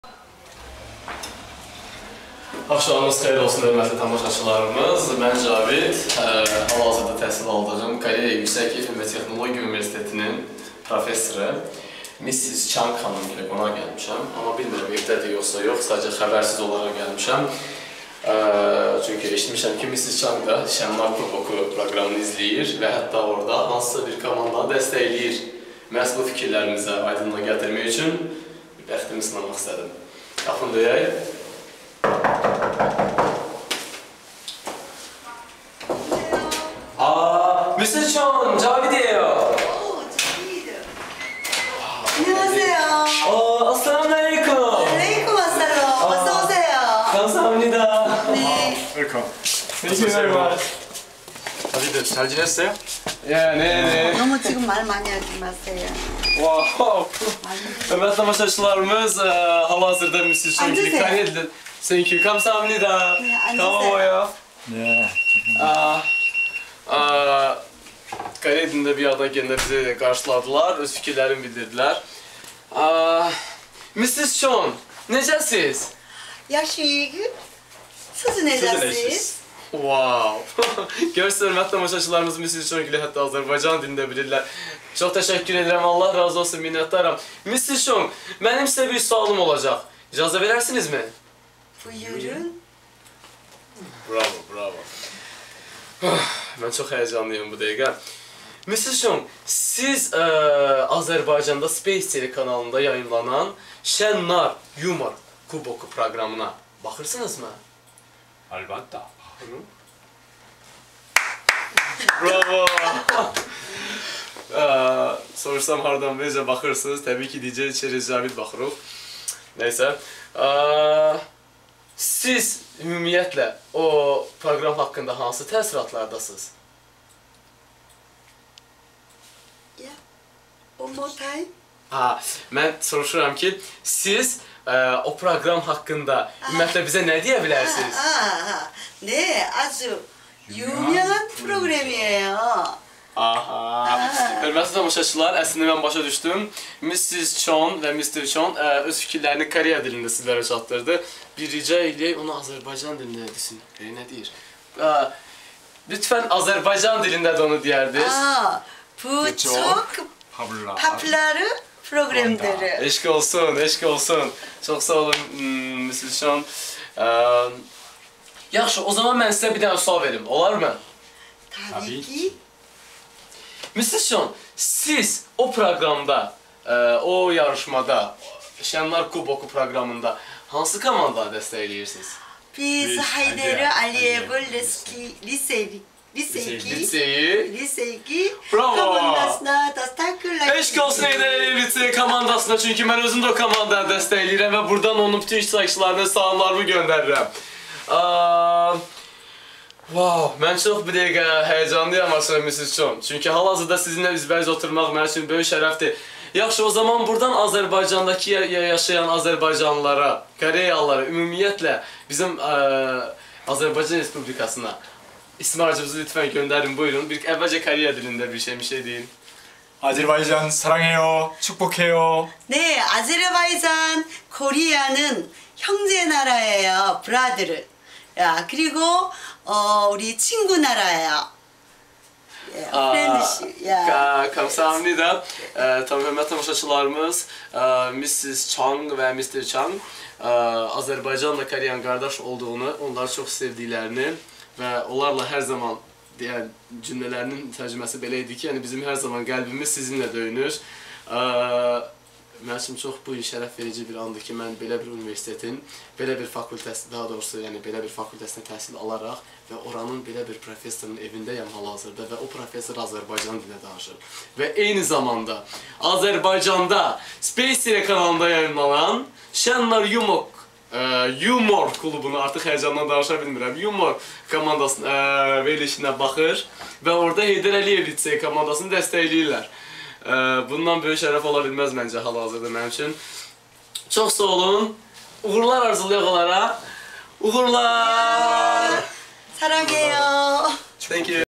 Javid Aliyev KAIST universitetinin professoru Mrs.Chang xanim gilde qonaq.MOV
Javid Aliyev Cenubi  Koreya Respulikasinda yerleshen KAIST(Korea Advanced Institute of Science and Technology) universitetinin professoru Mrs.Chang xanim gilde qonaq olub
Esas movzu Space TV'de yayimlanan shen NAR Kuboku verlishi aqqinda onlarin reyleri idi    axi onalr  bu verlishi sevev seve izleyirdiler ;P   Ugurlar))